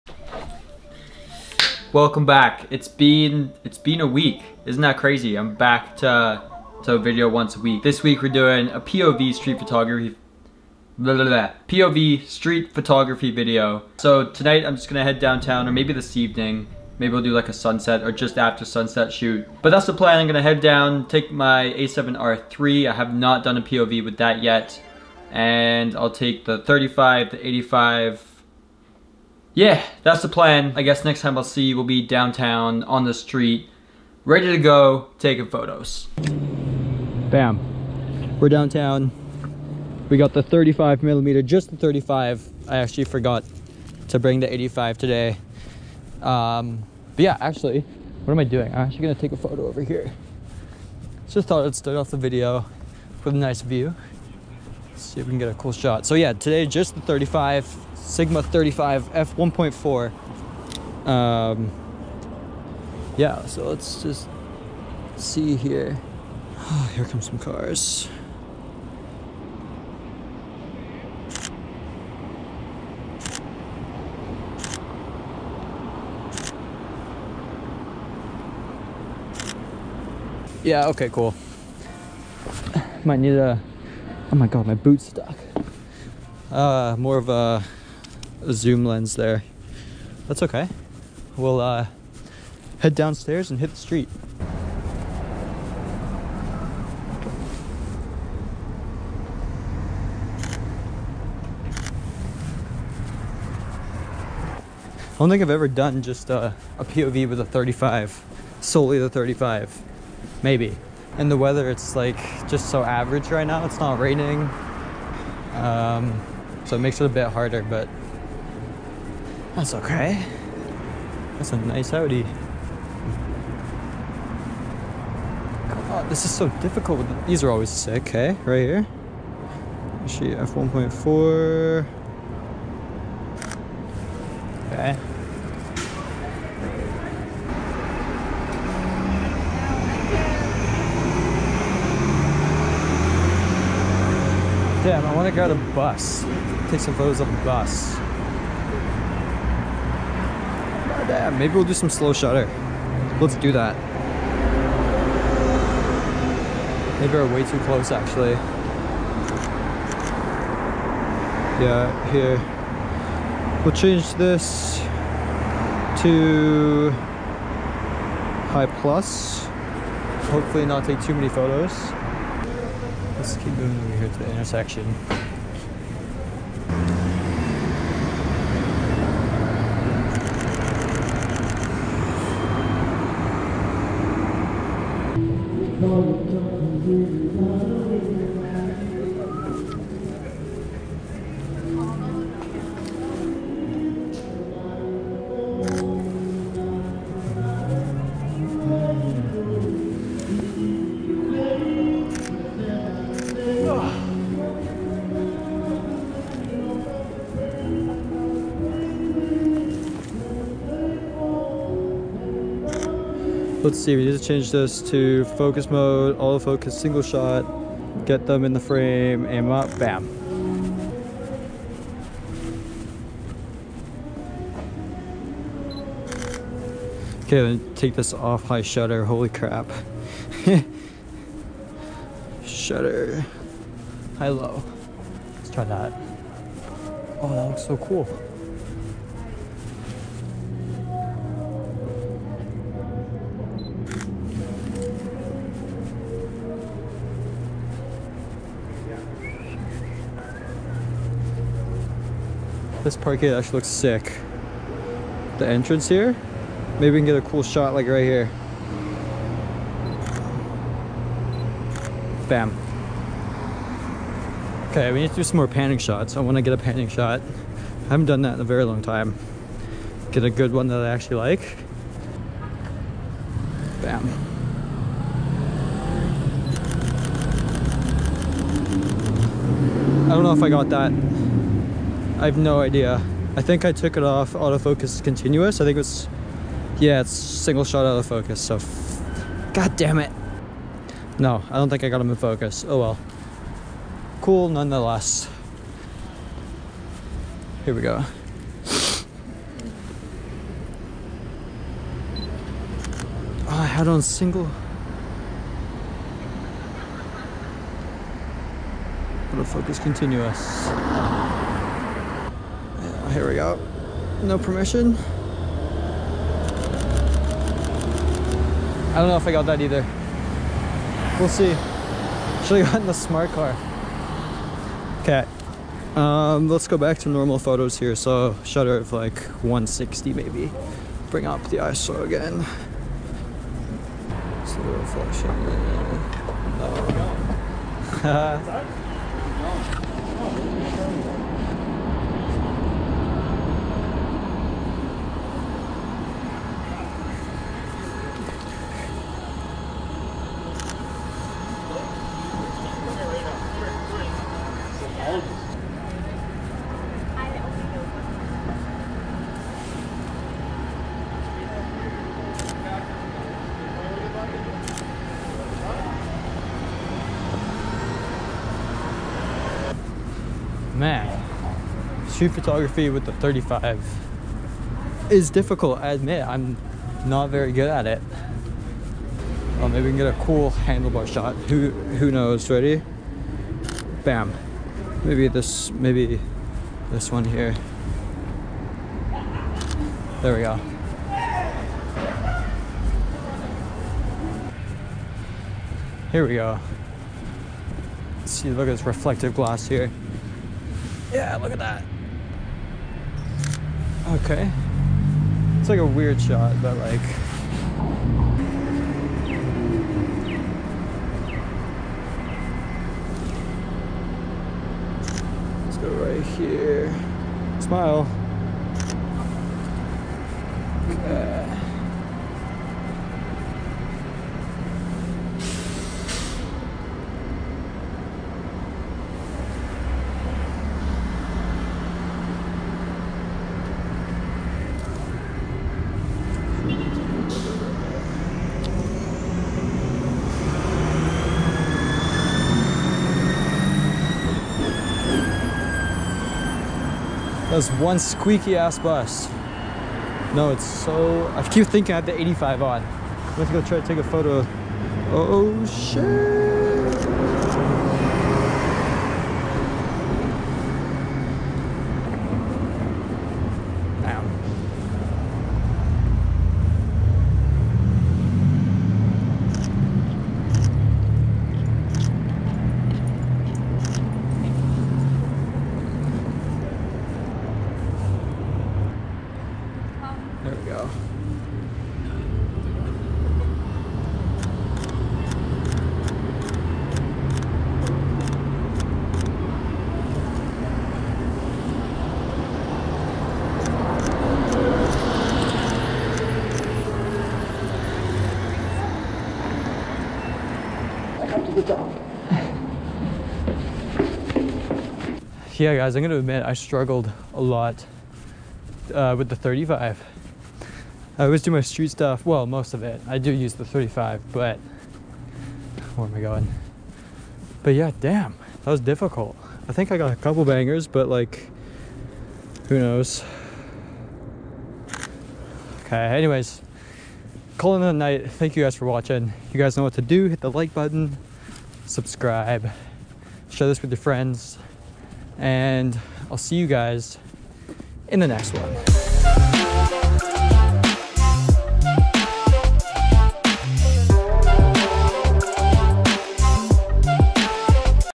35mm F1.4 POV Street Photography (night time)
Let me know your favorite photo👇  Thank you guys for watching and welcome to all the new subscribers (The channel is growing so fast its mind blowing)! The 35mm F1.4 lens from Sigma is a night time street photography beast and this video proves it haha. For anyone wondering this was shot in Victoria BC Canada. Let me know what you would like to see next and I'll see ya next week✌

My gear
Sony A7ii
Sony A7Sii
Sony A7Riii